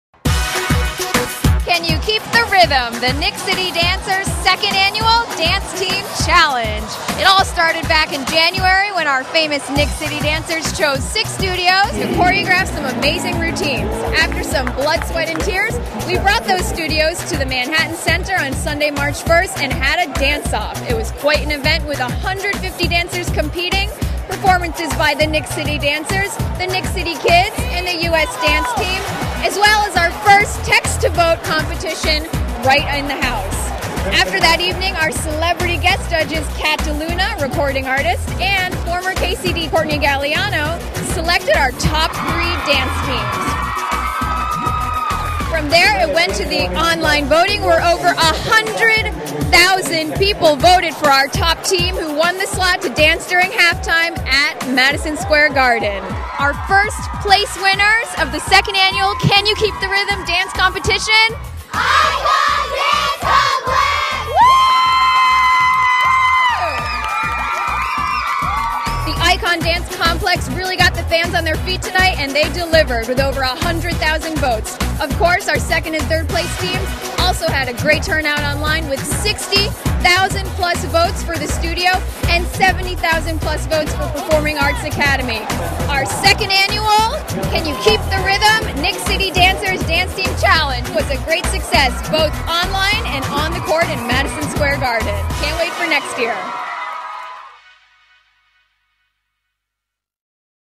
KCD "Can You Keep the Rhythm?" Dance Challenge
KCD "Can You Keep the Rhythm?" Dance Challenge Recap- April Wilkner hosts as Icon Dance Complex brings home the 1st place honors this year performing at Madison Square Garden during halftime of the March 18th Knicks vs Nets Game.